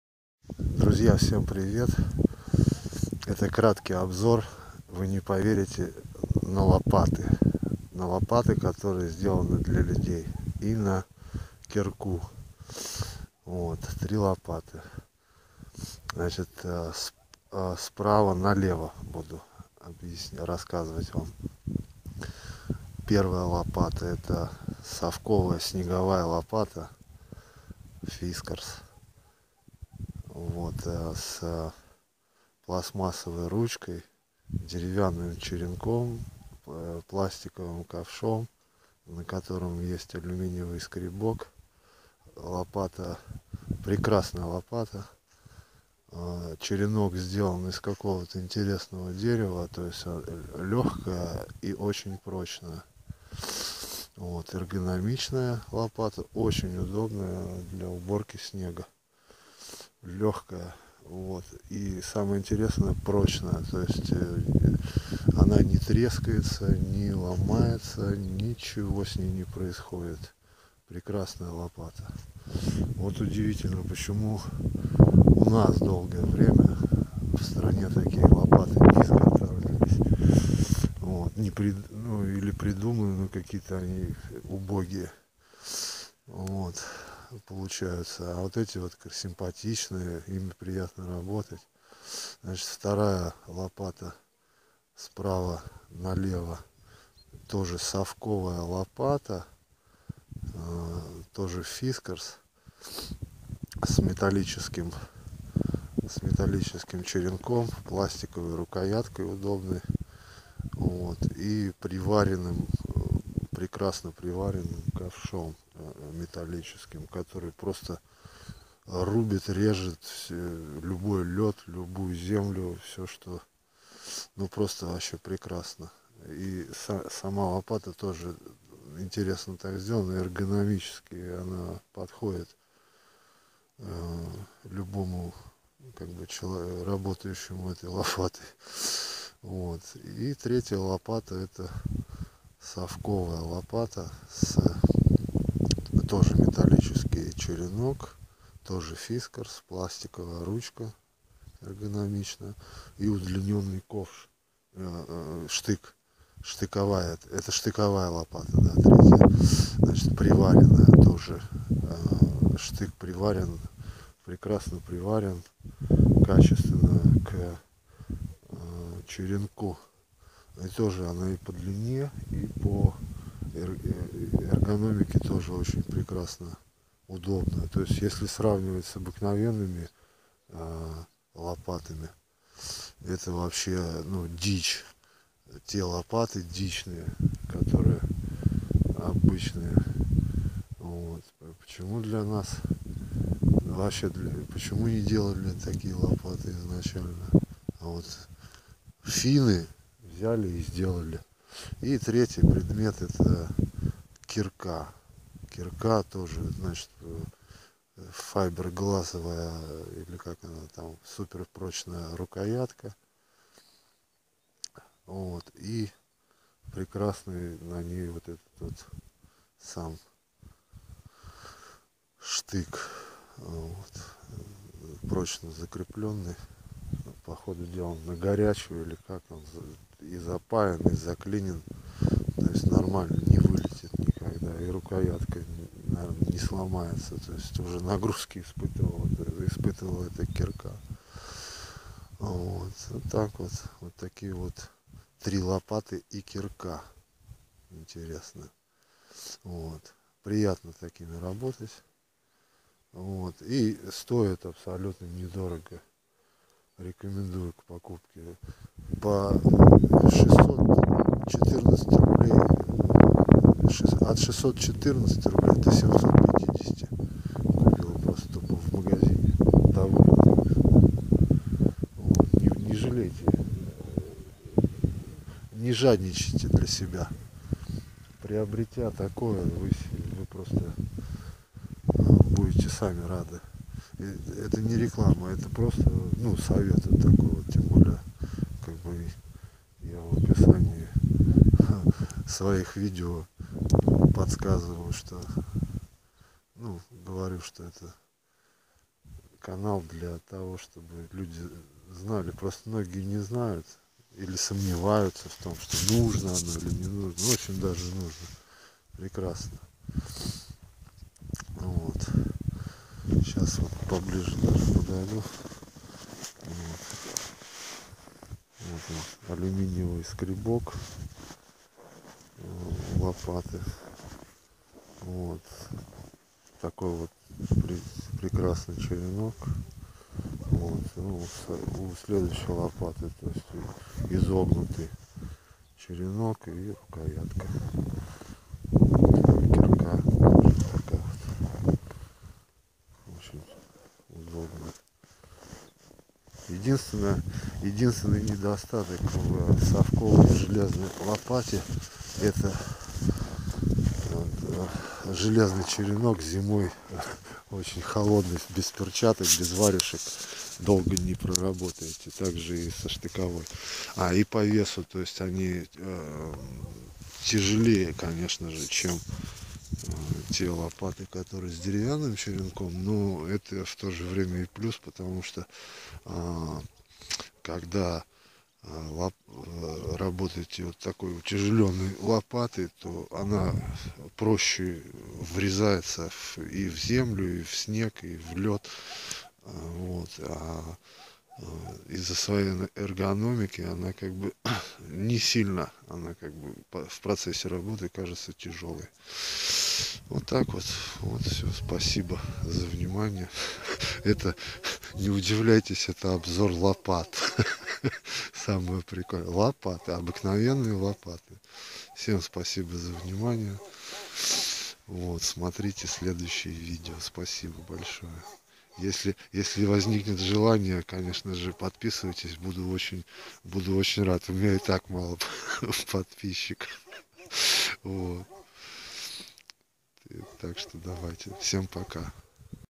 Финские лопаты для чистки снега и других работ, отличия от русских лопат. Fiskars лопаты для людей.
Вы не поверите, но это реально обзор на лопаты)))
Да, да! И такое бывает!!! ))))
это реально удобные лопаты ))
Пока сами не попробуете не поймёте...
Это не реклама, просто совет.
Вроде бы лопаты как лопаты , а вот нет, работается ими по чему то реально по другому - легче и веселее )) В чём тут фокус и загадка сам не пойму. ))
Наверное всё таки из за продуманной формы , веса , или ещё чего нибудь. ))